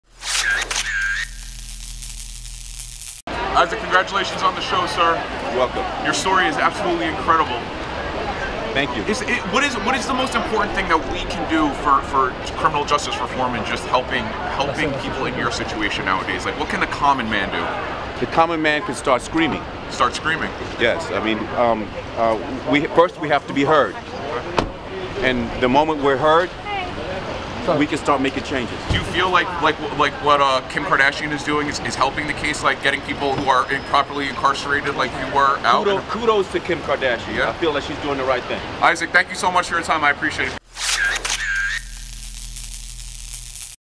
For Life's Prisoner-Turned-Lawyer Isaac Wright Jr. Praises Kim Kardashian | toofab
The KUWTK star's prison reform efforts have not gone unnoticed by someone all too familiar with the situation.